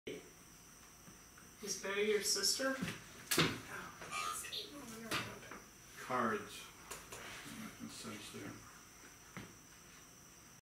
no4 red
While conducting an EVP session in the Doctor’s Office at the Lake of the Red Cedar's Museum in Cedar Lake, IN a question was asked, “Is Betty your sister?” A whispery voice in the background says, “No.”